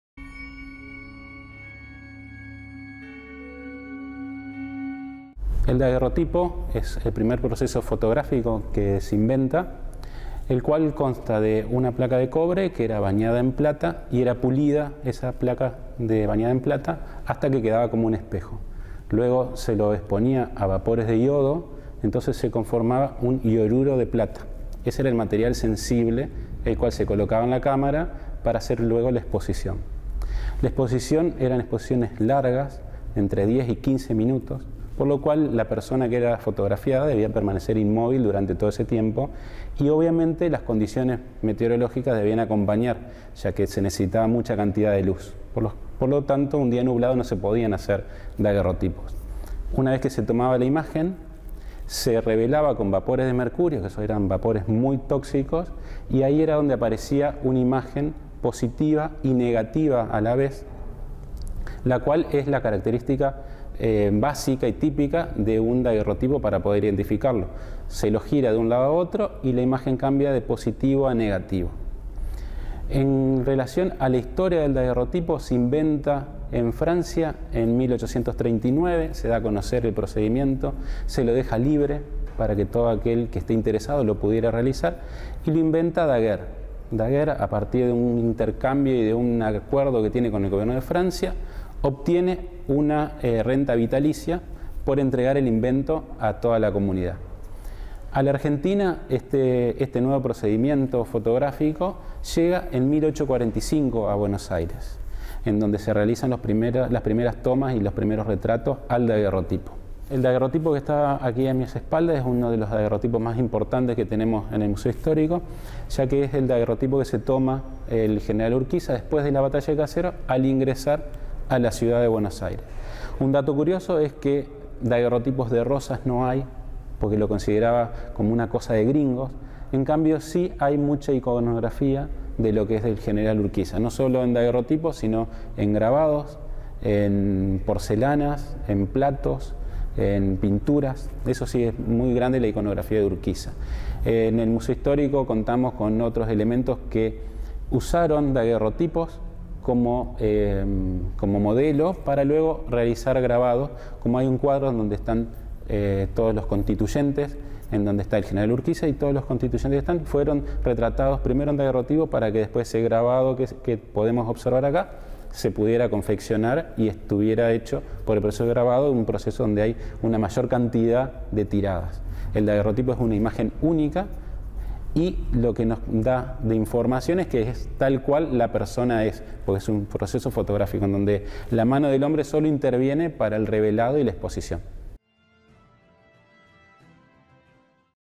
Daguerrotipo de J. J. de Urquiza
Entrevista al fotógrafo Federico García, curador del Museo Histórico Martiniano Leguizamón sobre los inicios de la fotografía y el daguerrotipo del Gral. J.J. de Urquiza que se encuentra en dicho museo entrerriano